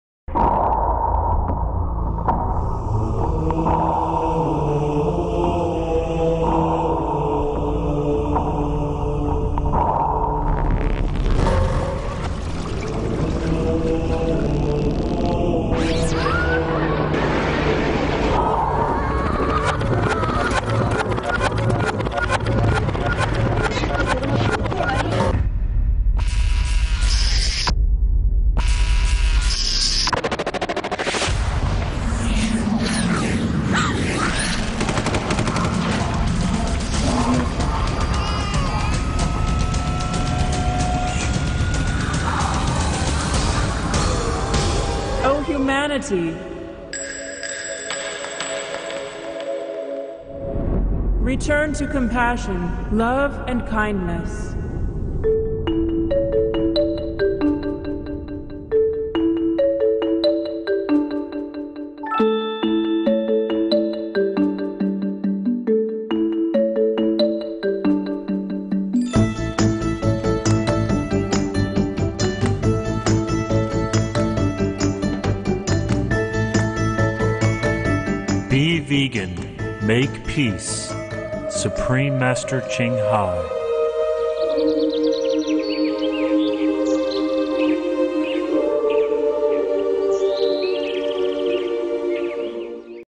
Awaken Humanity - Return to Compassion and LOVE!
❤ A moment for Human Earthlings to Awakening our Hearts and Souls!  One of the most self-empowering, compassionate, and environmentally sound actions we can take to awaken and raise our vibration mentally, emotionally, physically, and spiritually, is to no longer create and consume the trauma of other sentient beings.

Ⓥ Mother Nature is telling us that we are Vegan Beings (Plant-Based Diet) - epidemic levels of disease and environmental destruction!  At this point of our evolution, humans are biologically Herbivore/Frugivore

If we want Peace in our World - Hearts-Minds-Bodies-Souls - why would we create and consume violence? Thank you for being and/or becoming Vegan so ALL may LIVE!

VitalVeda claims no rights to this video or its contents - for educational and enlightenment purposes only.  For information on how you can save our Planet and ALL Life by embracing a Vegan Life